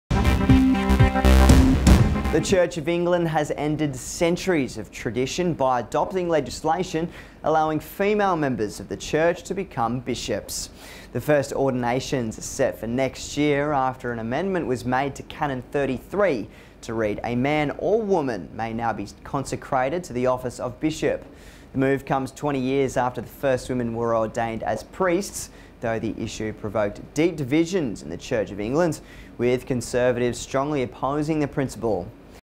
Church Of England Allows Women Bishops - Nov 19, 2014
The Church of England has ended centuries of tradition by adopting legislation allowing female members of the church to become bishops. The first ordinations are set for next year, after an amendment was made to Canon 33 to read “a man or woman may now be consecrated to the office of bishop”. The move comes 20 years after the first women were ordained as priests, though the issue provoked deep divisions in the Church of England, with conservatives strongly opposing the principle.